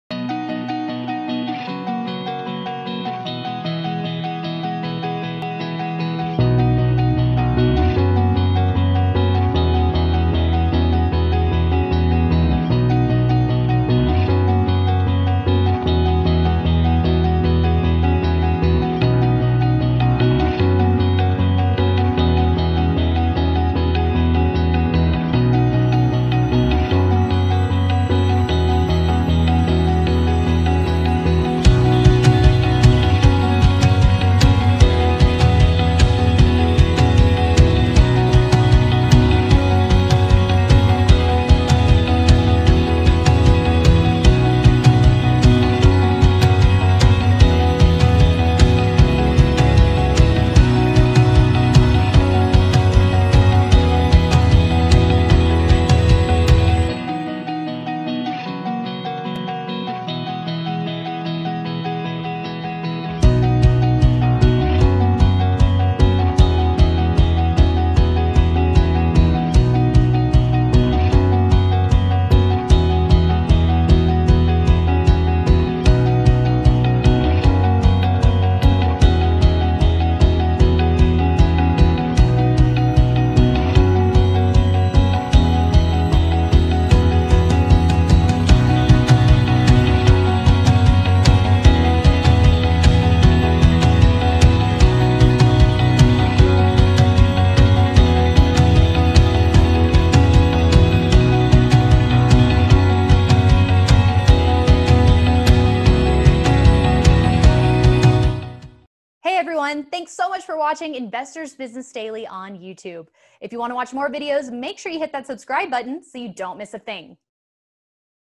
Valentine's Day Financial Do's And Don'ts For Happy Couples
Debt.com chairman Howard Dvorkin says, "Fighting about finances is the number one reason for divorce."